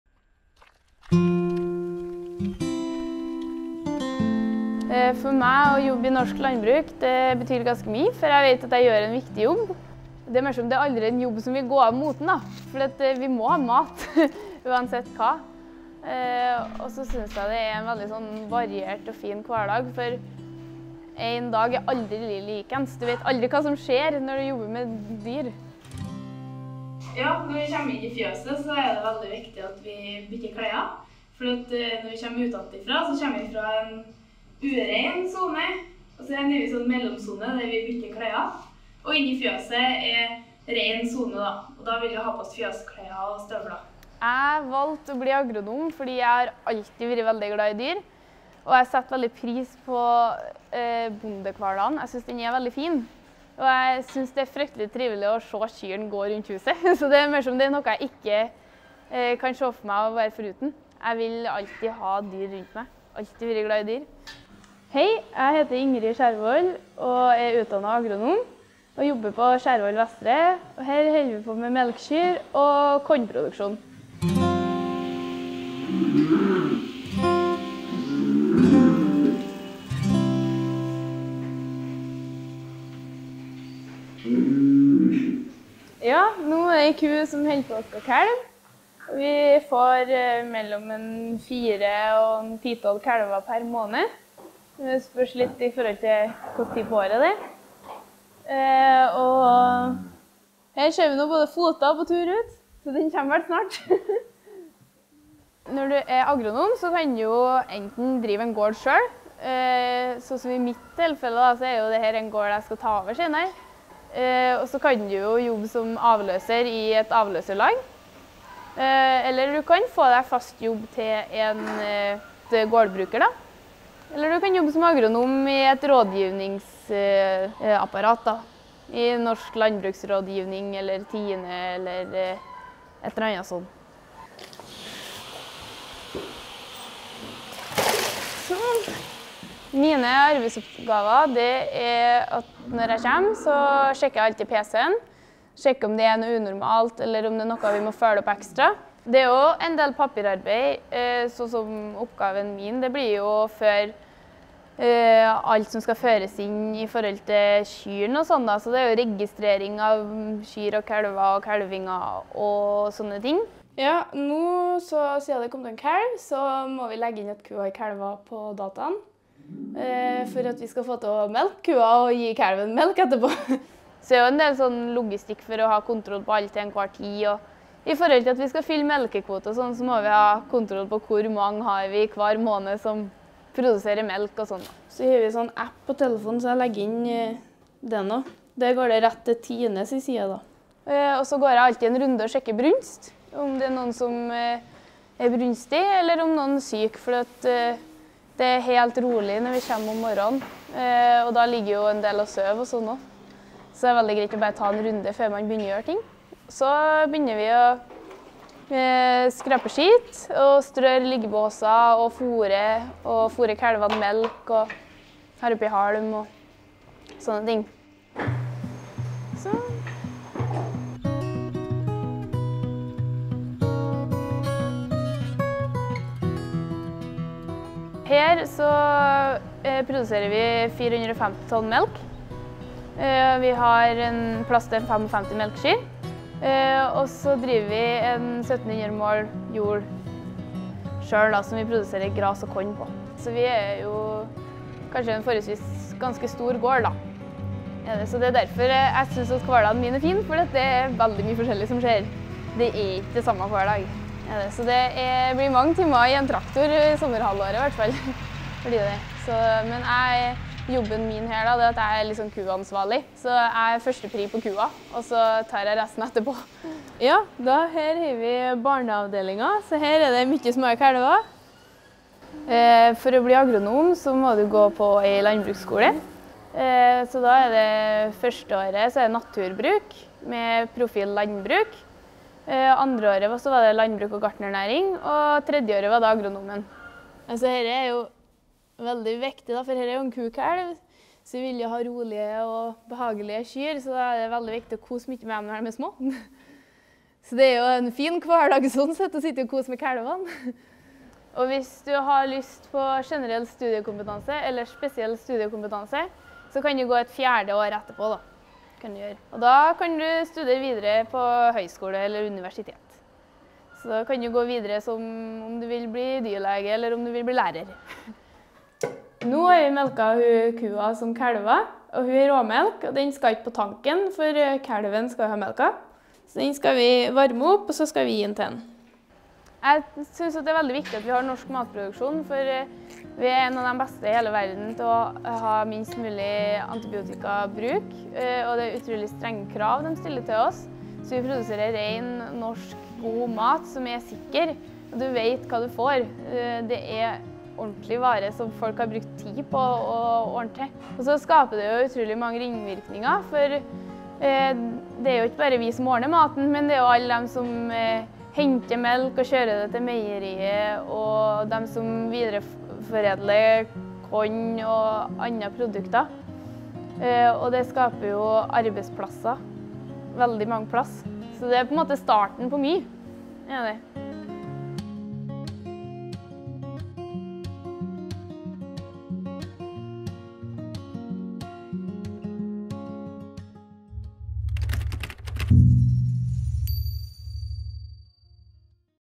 Yrkesportrett: agronom
En agronom jobber med husdyr og/eller planteproduksjon på en landbrukseiendom. Agronomer kan enten drive egen gård eller være ansatt innen landbruket. Arbeidshverdagen til en agronom består av mange forskjellige praktiske arbeidsoppgaver.